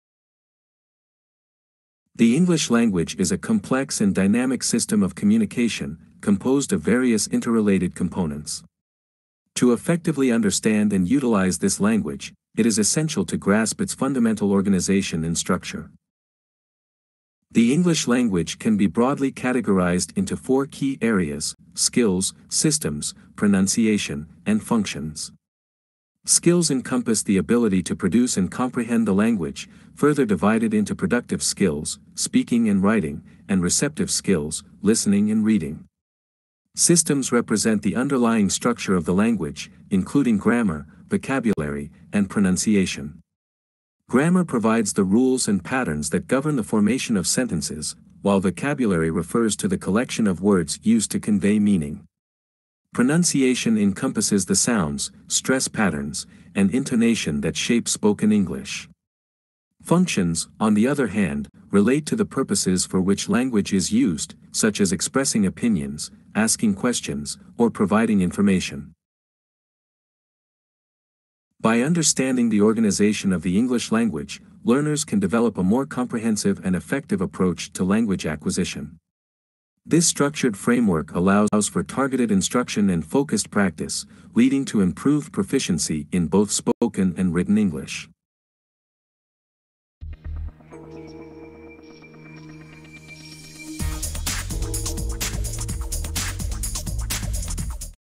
Aula de inglês: ENGLISH Skills and Systems
🔘RuiEnglish Lec: ENGLISH Skills and Systems

The English language is a complex and dynamic system of communication, composed of various interrelated components. To effectively understand and utilize this language, it is essential to grasp its fundamental organization and structure.

The English language can be broadly categorized into four key areas: skills, systems, pronunciation, and functions. Skills encompass the ability to produce and comprehend the language, further divided into productive skills (speaking and writing) and receptive skills (listening and reading). Systems represent the underlying structure of the language, including grammar, vocabulary, and pronunciation. Grammar provides the rules and patterns that govern the formation of sentences, while vocabulary refers to the collection of words used to convey meaning. Pronunciation encompasses the sounds, stress patterns, and intonation that shape spoken English. Functions, on the other hand, relate to the purposes for which language is used, such as expressing opinions, asking questions, or providing information.

By understanding the organization of the English language, learners can develop a more comprehensive and effective approach to language acquisition. This structured framework allows for targeted instruction and focused practice, leading to improved proficiency in both spoken and written English.